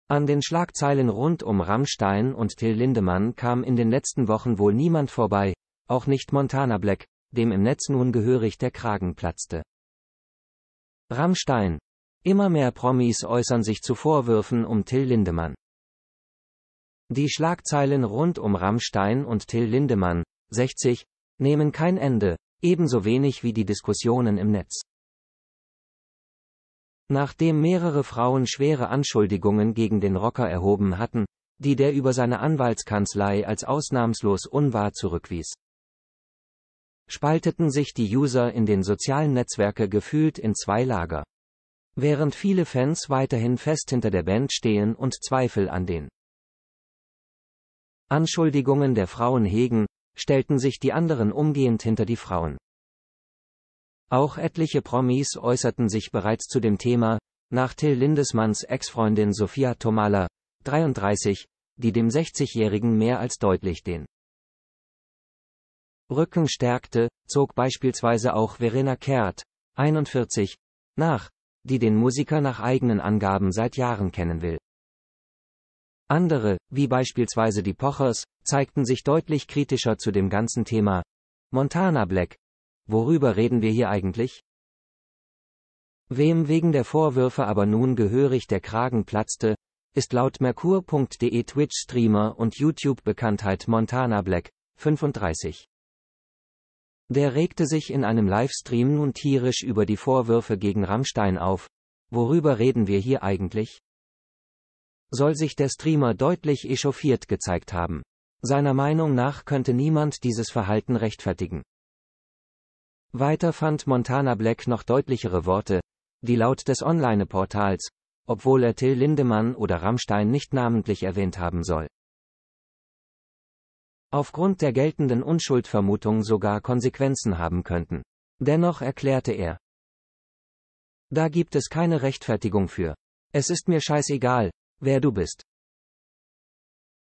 MontanaBlack Heftiger Wutausbruch im Netz wegen Rammstein!
An den Schlagzeilen rund um Rammstein und Till Lindemann kam in den letzten Wochen wohl niemand vorbei - auch nicht MontanaBlack, dem im Netz nun gehörig der Kragen platzte!